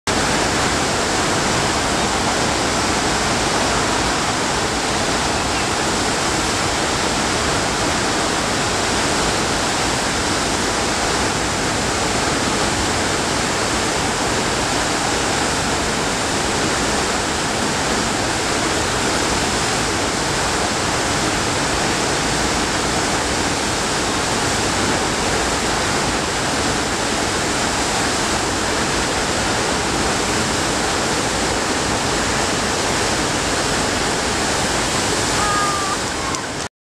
Flowrider Surfing
My first morning trying surfing on the Flowrider @ the Kalahari Resort, OH.